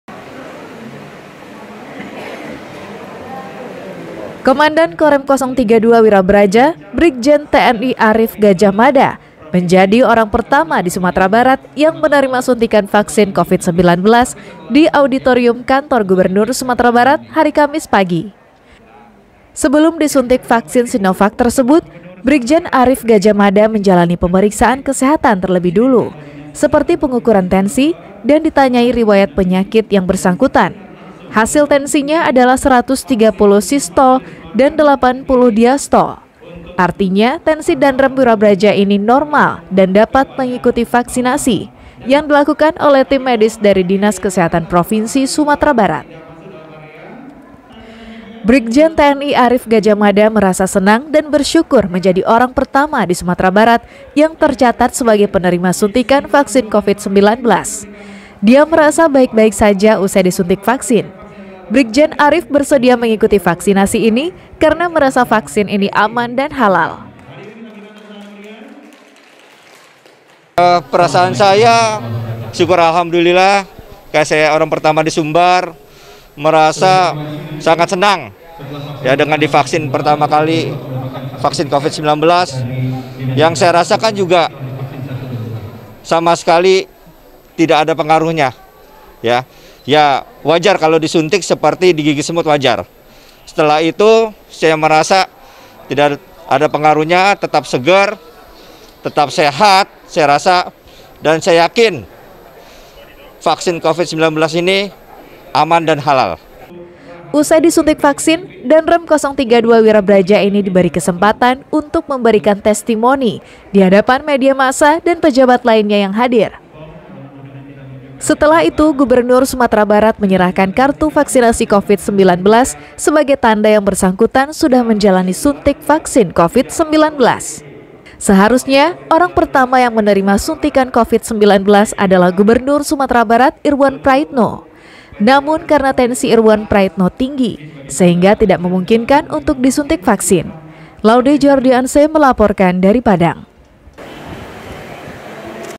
DANREM 032 WIRABARAJA MENJADI ORANG PERTAMA DI SUMATERA BARAT YANG DISUNTIK VAKSIN COVID-19
PENGAKUAN DANREM, VAKSIN PENTING KARENA AMAN DAN HALAL